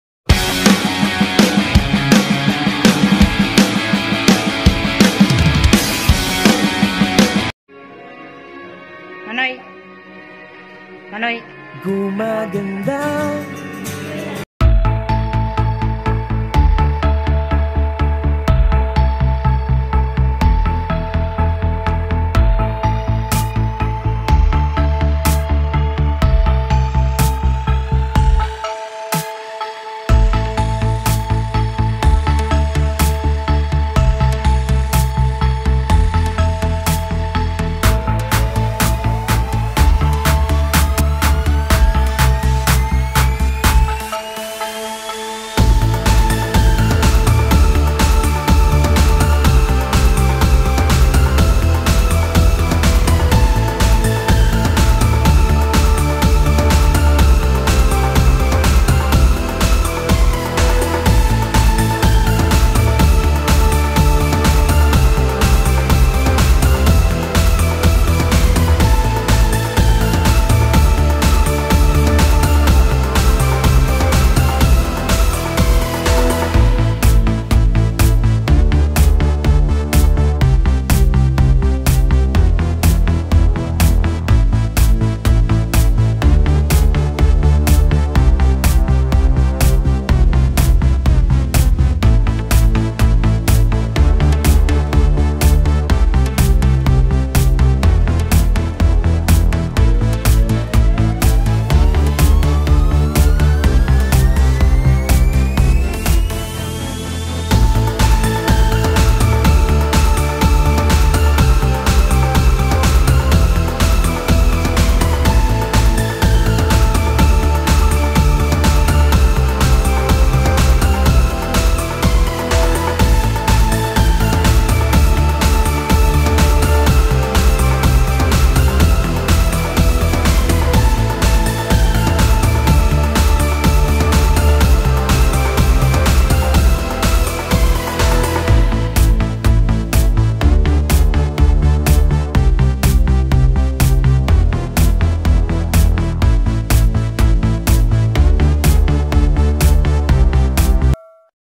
FAMILY BONDING | MT.MAYON REST HOUSE
Salamat sayo tita men sa pag setup ng inyong pamamasyal na kasama si papa at si lola na kahit papano nag bonding kayo mgkakapait kahit hindi kayo kompleto.

Photograph by: Carmen Bonagua